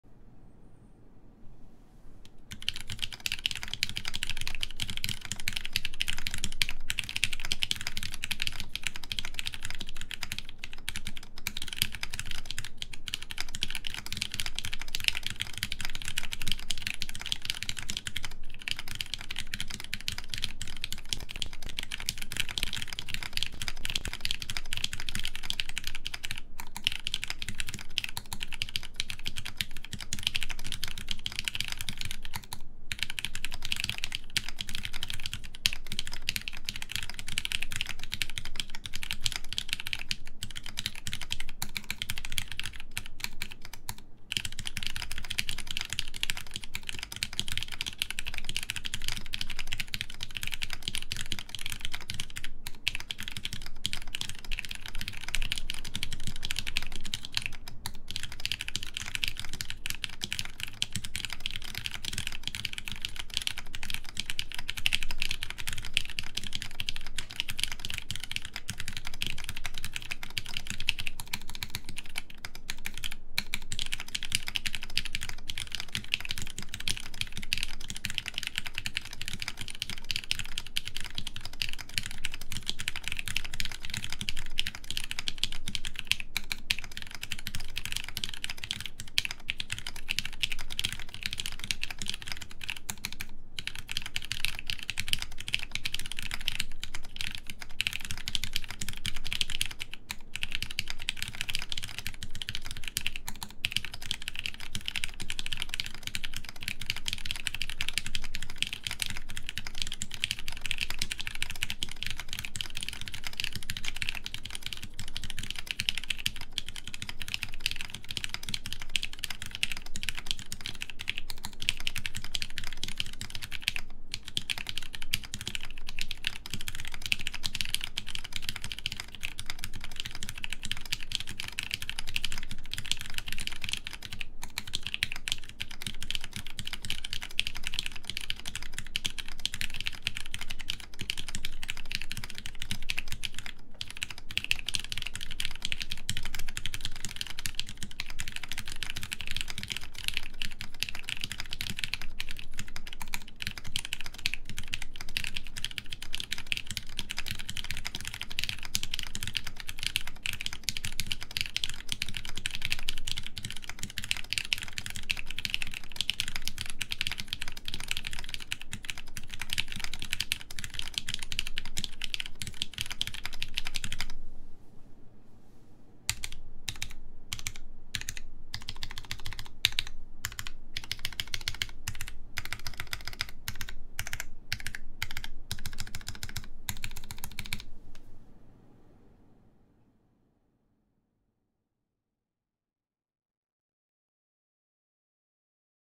TGR x Linworks Dolice with lubed Hirose Orange Switches Typing Sounds
Designer: TGR and Linworks
Keyboard: Dolice (F/F Dolch colorway)
Plate: aluminum full
Keycaps: GMK Carbon R1, Umka
Switches: Cherry MX Hirose Orange
Lube: Krytox 205g0
Springs: 55g SPRiT
Switch Films: TX films
Stabilizers: Screw-in Durock v2 stabilizers, lubed with 205g0

Camera: Sony a6400 with Sony 18-135mm lens
Interface: RME Babyface Pro
Microphone: RØDE NT1
Setting: Default settings @ 44.0dB gain
Post: slight base audio level / gain adjustment